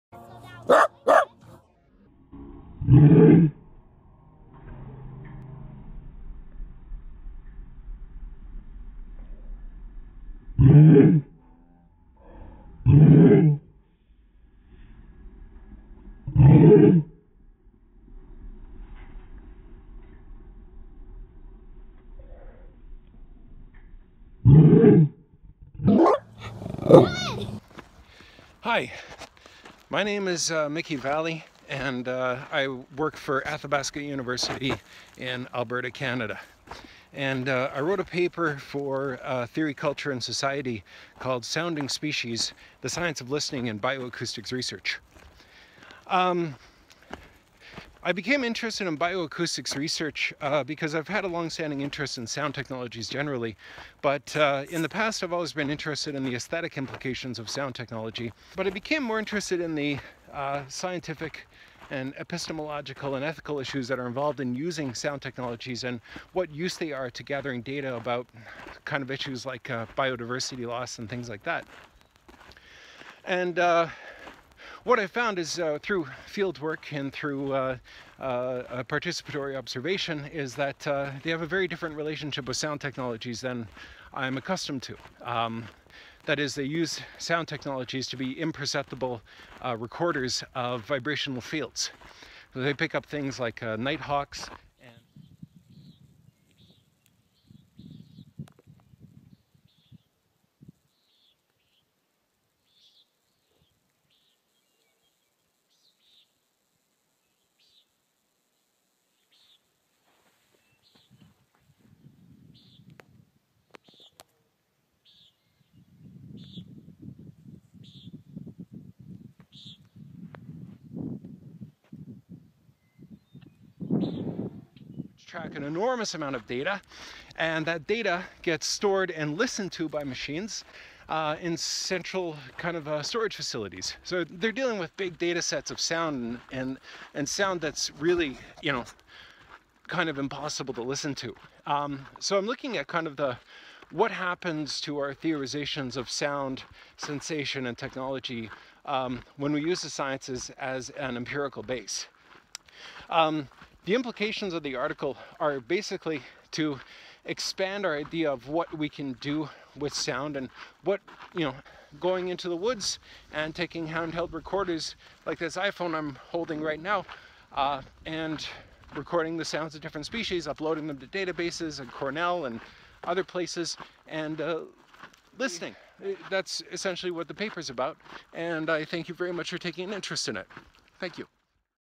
Mickey Vallee on 'The Science of Listening in Bioacoustics Research: Sensing the Animals' Sound'
Mickey Vallee introduces his Theory, Culture & Society article 'The Science of Listening in Bioacoustics Research: Sensing the Animals' Sound'

Abstract
Bioacoustics is an interdisciplinary field bridging biological and acoustic sciences, which uses sound technologies to record, preserve, and analyse large datasets of animal communications. But it is also a world, made of the meanings created through inter- and intra-species communication. This article empirically explores a variety of bioacoustics research, including interviews with researchers, as part of a broader qualitative study, in order to theorize the expanding sense and sensation of a global biosphere and sonic data. By giving a sustained and detailed account of the science of bioacoustics, particularly how its modes of measurement allow for a new way of understanding what is involved in the de-centred modes of hearing that re-centre acts of listening and, by extension, the nature of the relation between researcher and researched, the article contributes to methodological discussions regarding the longstanding questions of how researchers and scientists are implicated in the knowledge and objects they collectively produce.